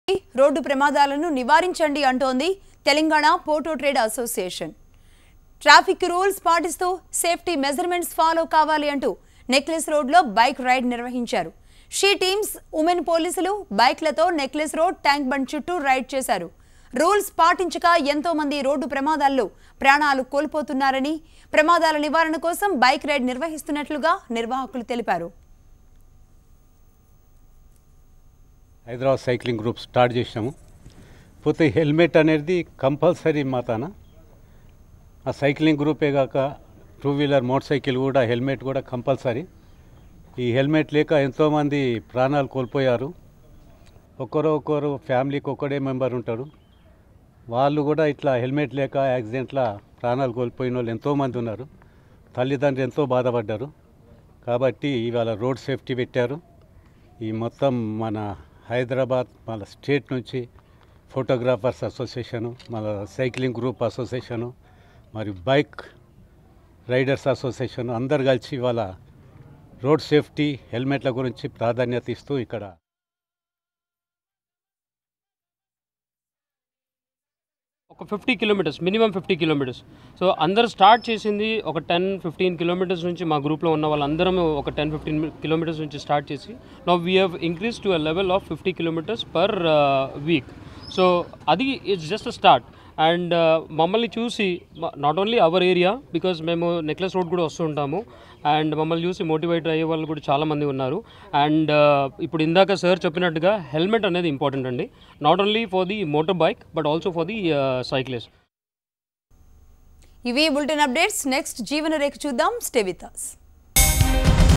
Telangana Photo Trade Association Conduct 2k Run In Necklace Road | HMTV
HMTV is the most credible Telugu News channel. Watch HMTV News Channel for all Latest Statewide, National, and International News.